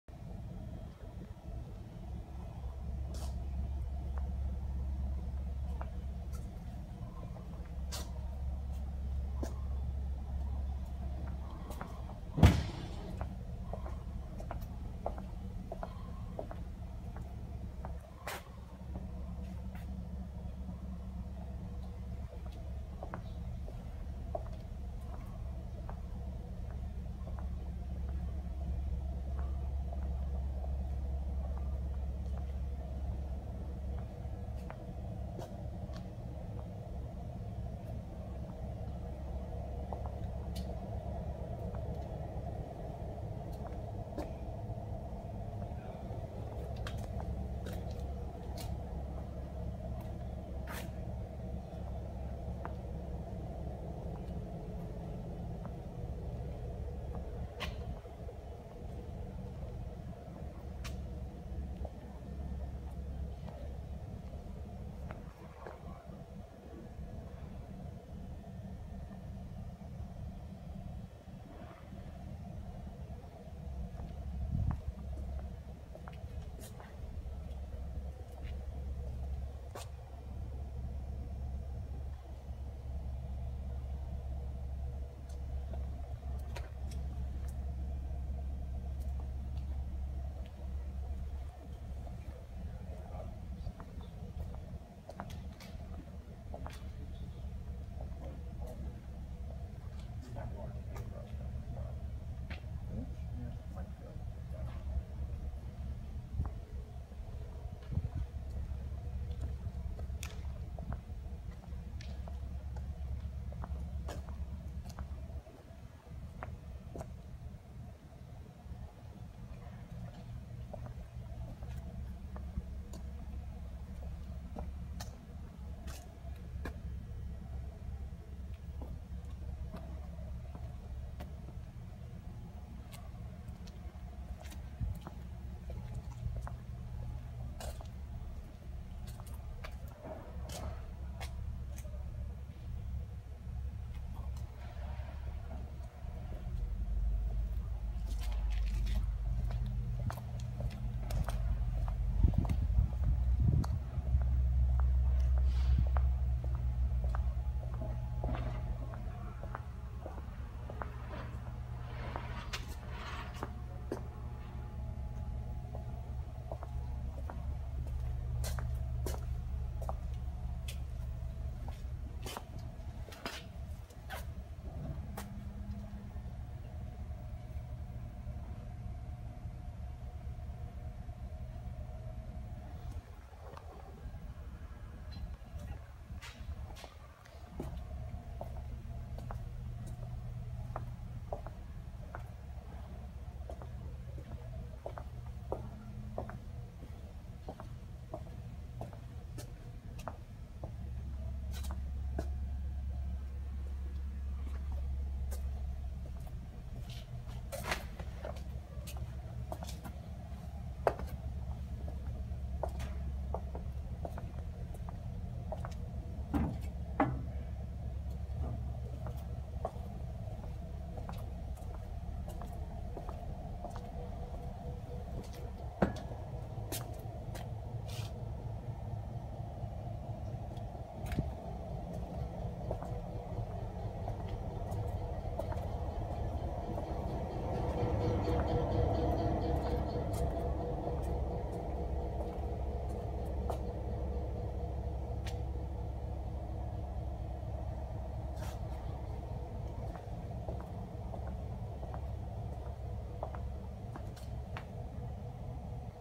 4.5" (114mm) x .237" (6mm) Yoder Rafted Stainless Steel Tube Mill
4.5" (114mm) x .237" (6mm) Yoder Rafted Stainless Steel Tube Mill with Inline Bright Annealing, Our Stock Number 113024
Must see, Pristine Condition!